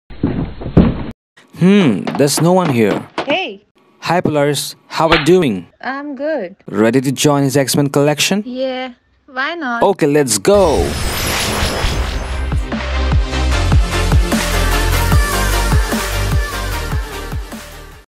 X-Men | Havok and polaris | Marvel legends
havok polaris added into my xmen team of Marvel legends

special thanks to @abstractashart and @shraddhajadhav for their amazing voices